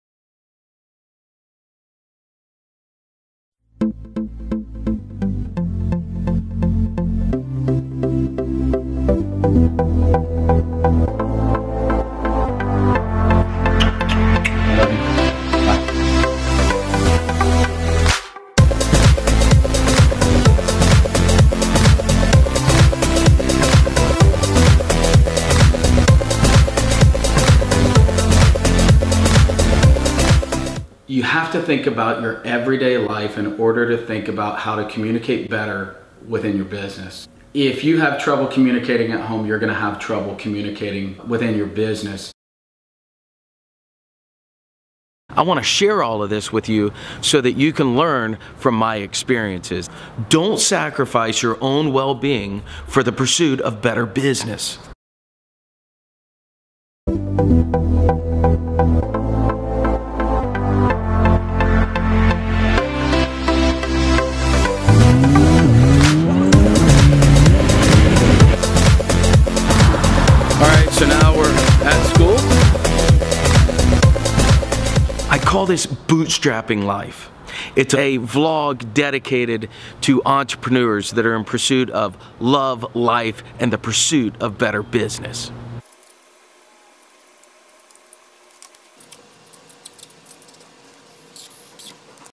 Cassidy Phillips - An Entrepreneurs Vlog - Trailer
This video is a trailer for my new YouTube channel,  Entrepreneurs guide For Love, Life and the pursuit of Better Business.

My name is Cassidy Phillips, I founded TriggerPoint Performance over 13 years ago, I've done over $60 million in revenue and traveled the world.  I want to share all of my experiences with you.

I've been featured in publications and have motivated and inspired people around the world.

I had huge success but I made mistakes along the way. I now have time in my life that I can share those experiences with you so that you don't have to make  the same mistakes that I made.

You only live once work hard to play hard!!